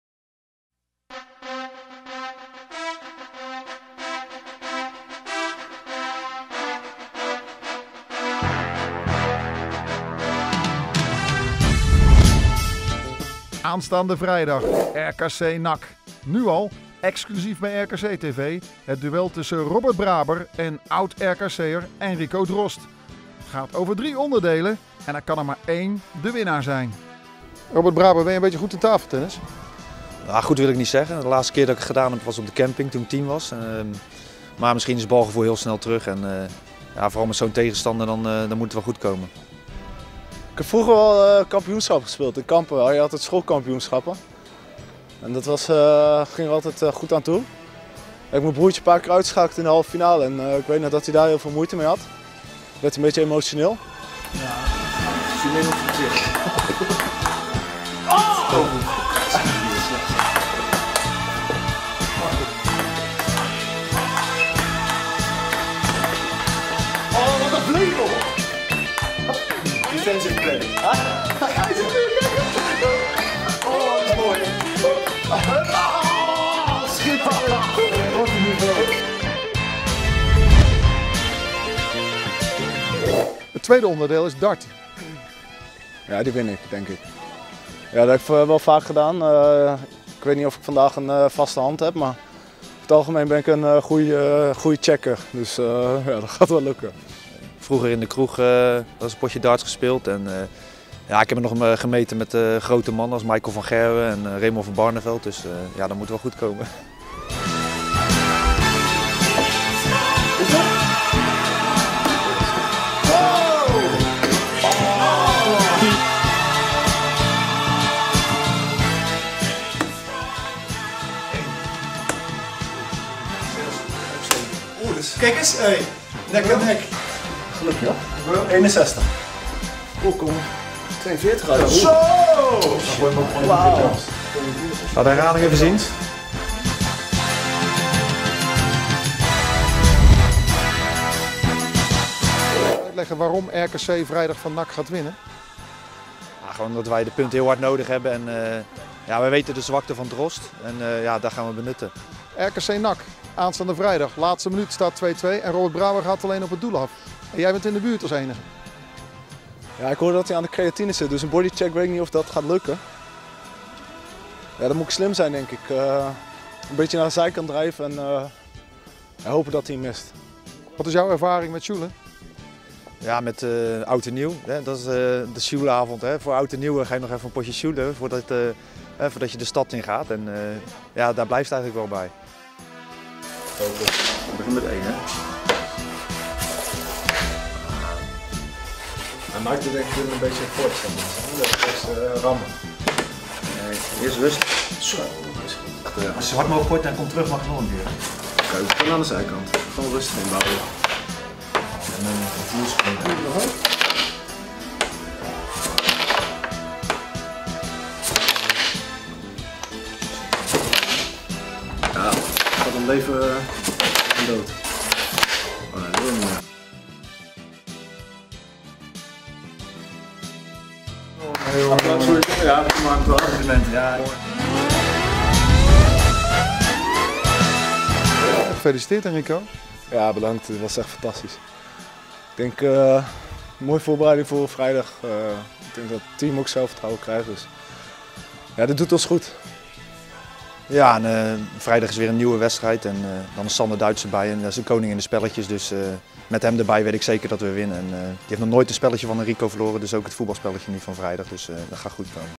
RKC TV: Op pad met Robert Braber en Henrico Drost
In aanloop naar de wedstrijd RKC WAALWIJK - NAC Breda ging RKC TV op pad met middenvelder Robert Braber van RKC WAALWIJK en verdediger Henrico Drost van NAC Breda.